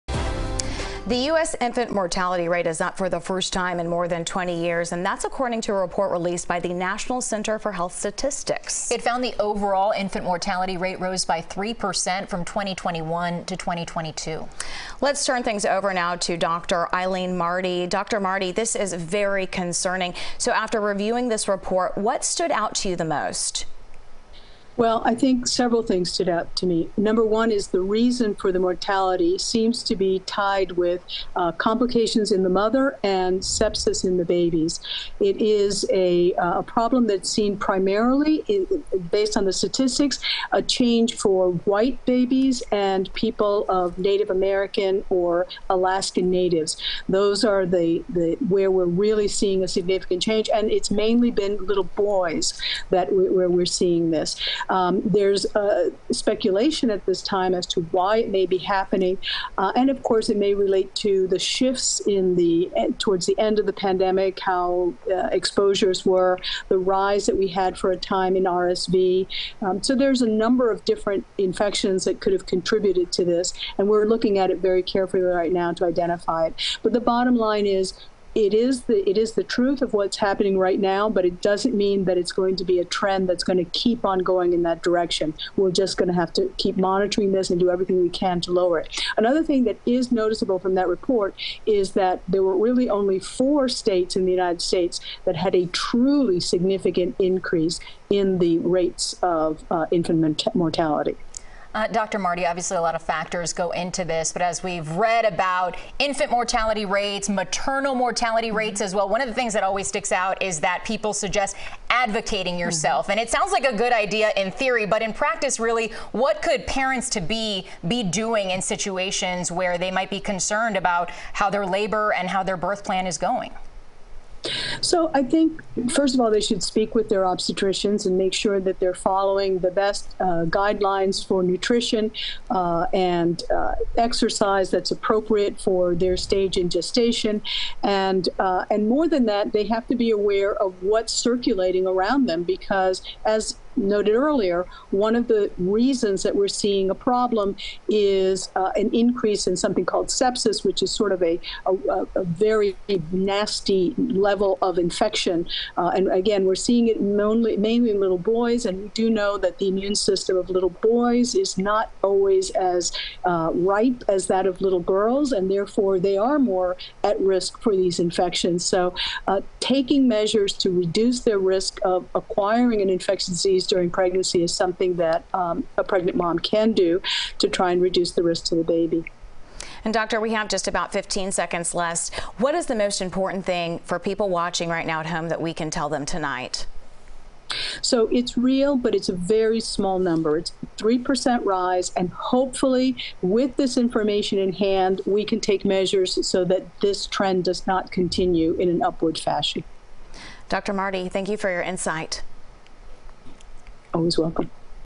US infant mortality rate up for first time in 20 years
Dr. Aileen Marty, FIU professor, expert in infectious disease and disaster medicine on infant mortality rates in the US.